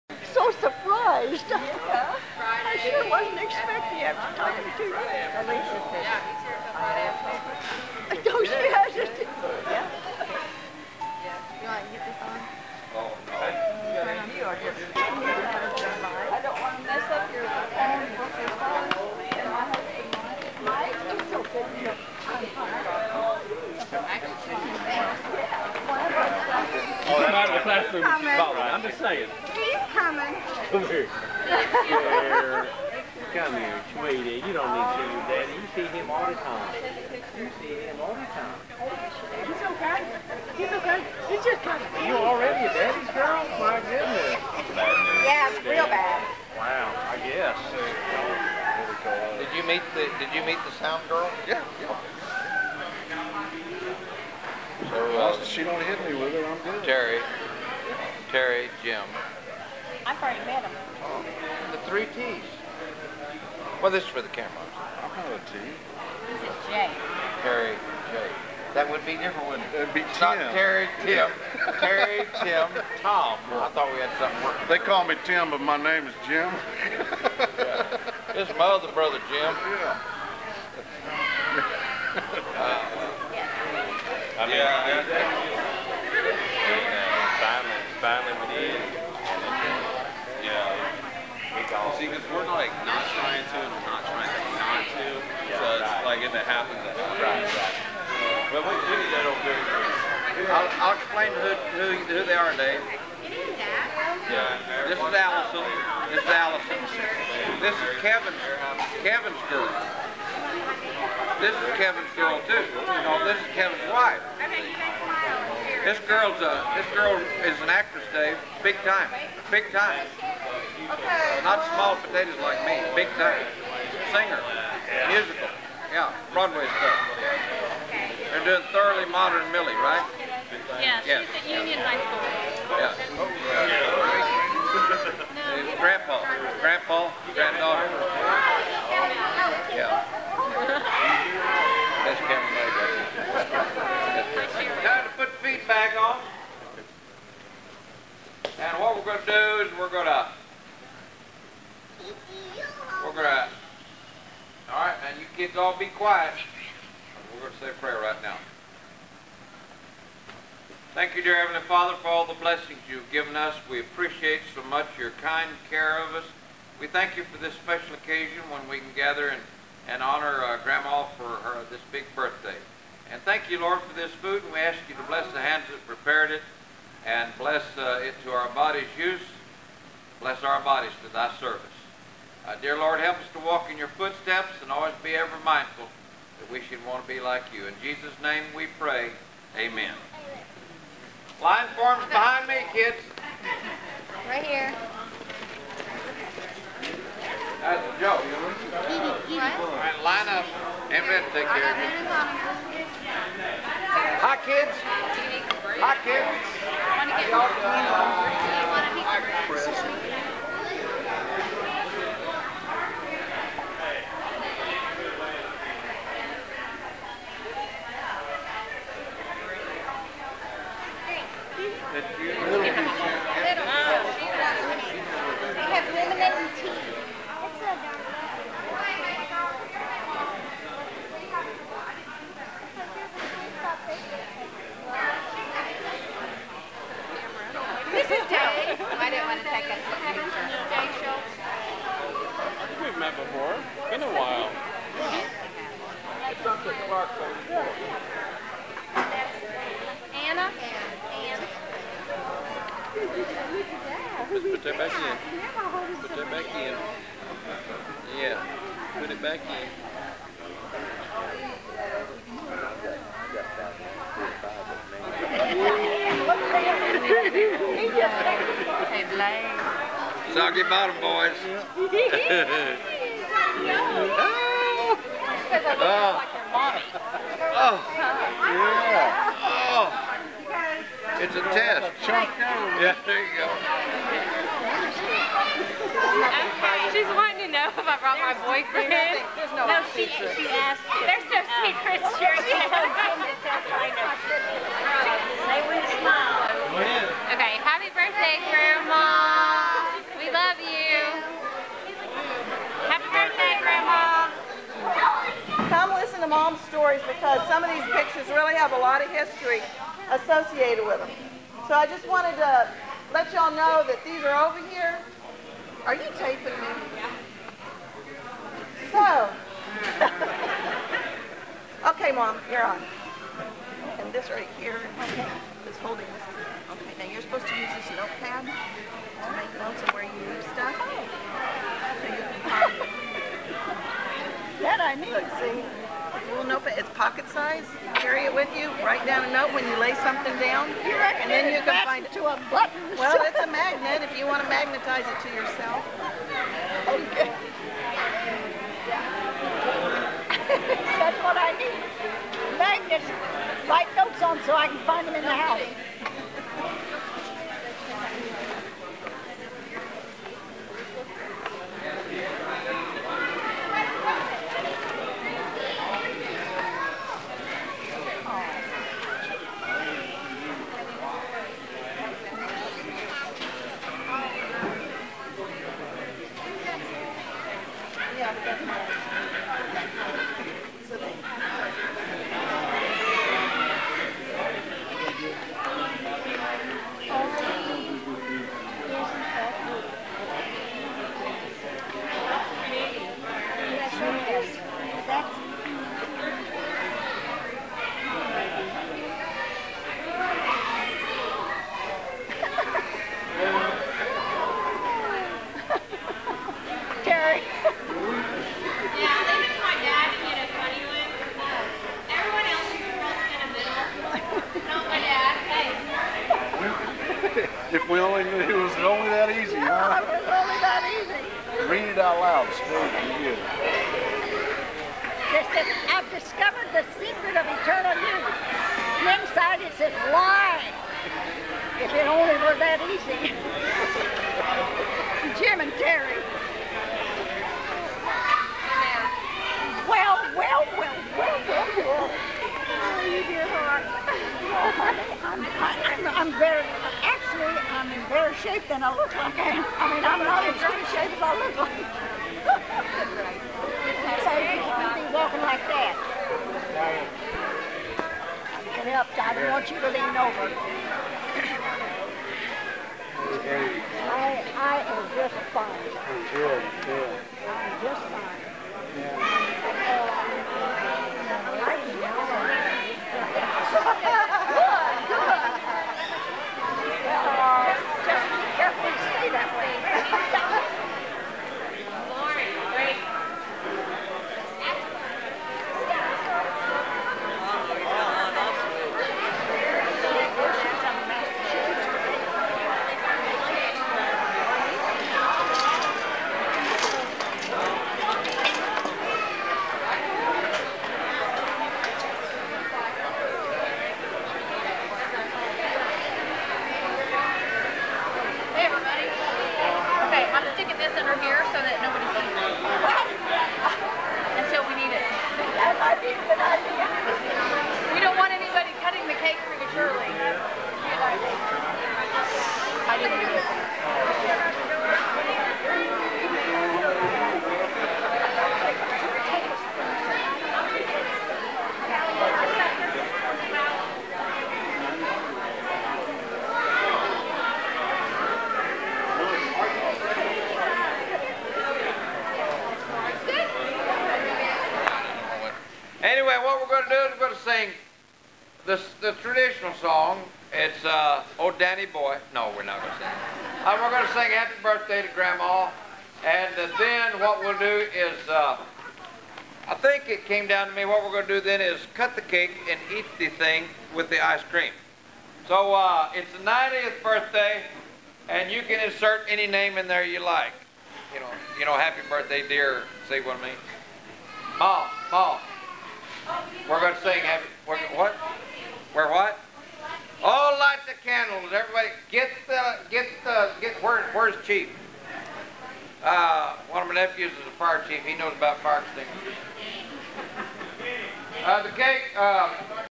Part 1 - Norma Ward Birthday - 90 Year Old Great Grandmother Trick Roping
Norma Ward celebrates her 90th birthday with family and friends in Guthrie, Oklahoma by trick roping like she has since 1930.  The actual trick roping is in part two.